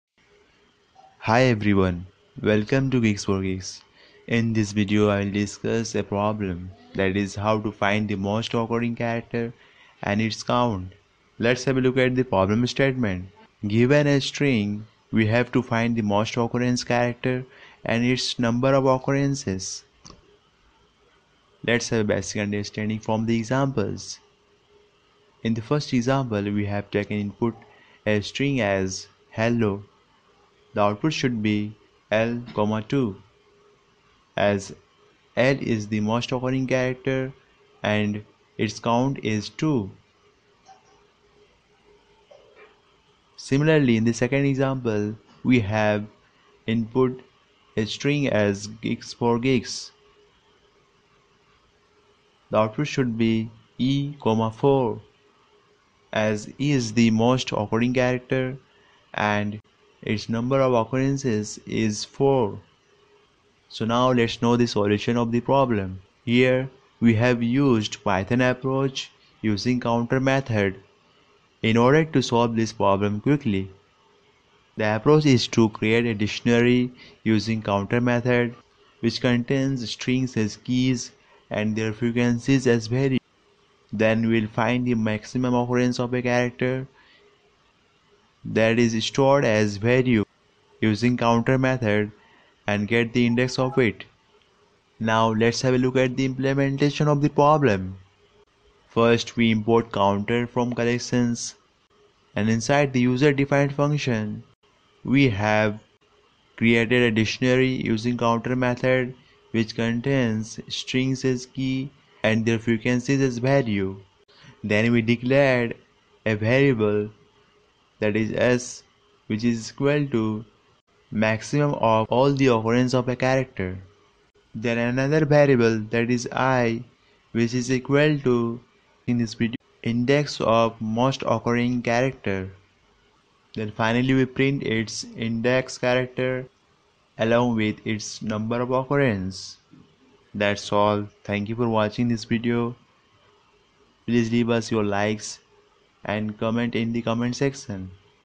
Python Programming Tutorial | Find the most occurring character and its count | GeeksforGeeks
This video is contributed by Afzal Ansari